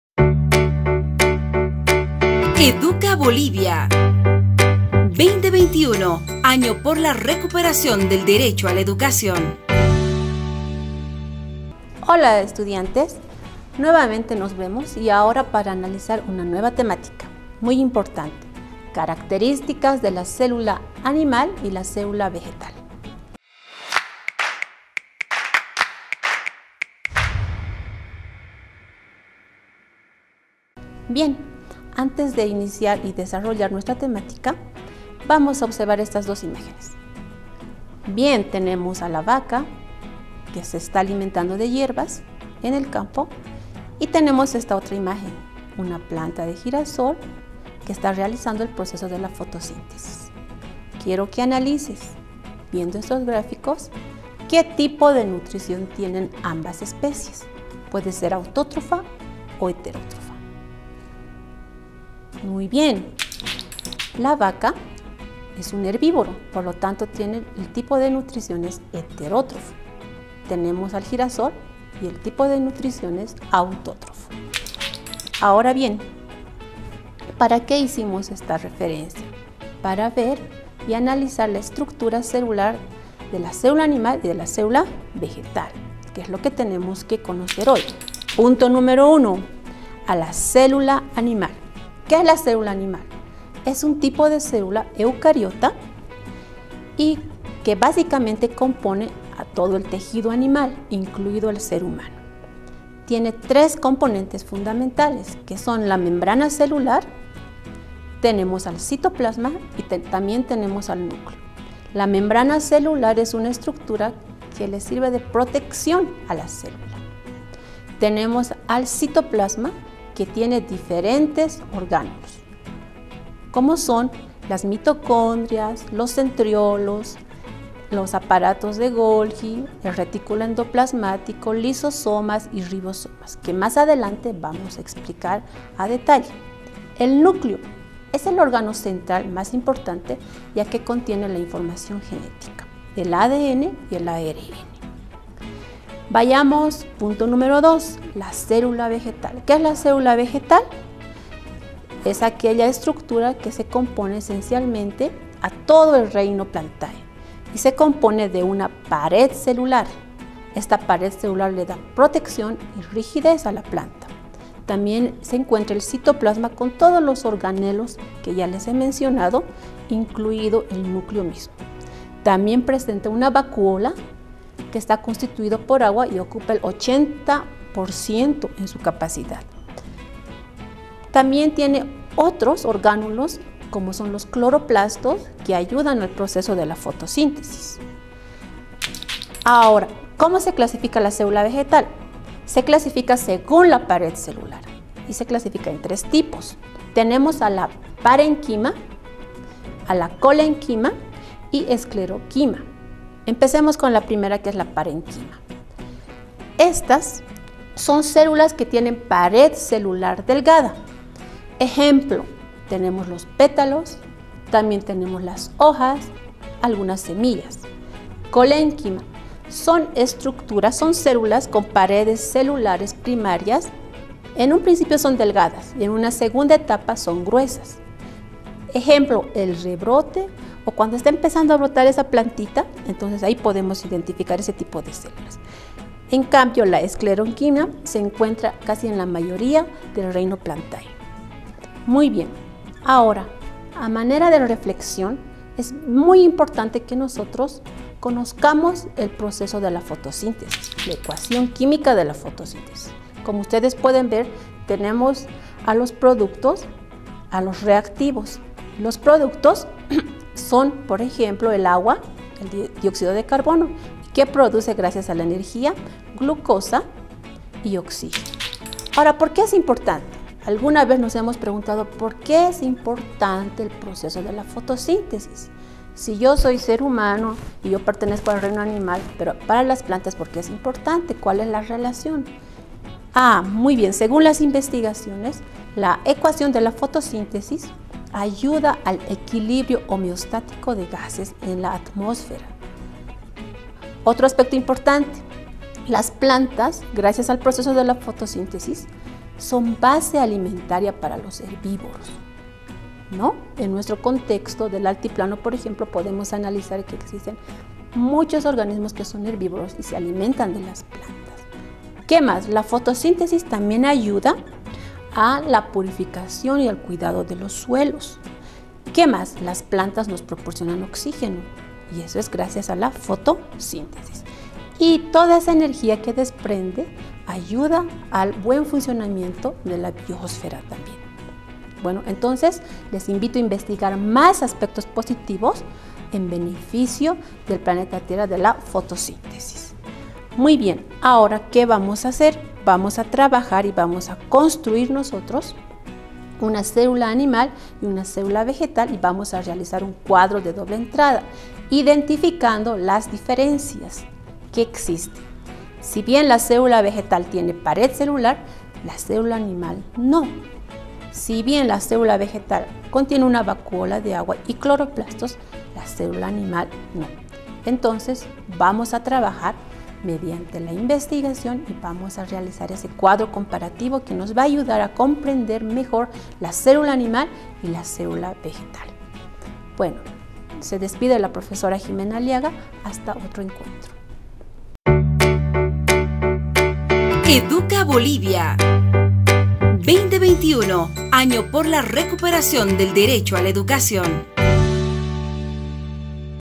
6to Año de Educación Secundaria Comunitaria Productiva – Biología Geografía
6to Año de Educación Secundaria Comunitaria Productiva – Biología Geografía
“Características de la célula animas y vegetal” con la Maestra Ximena Aliaga
Clase viernes, 19 de febrero de 2021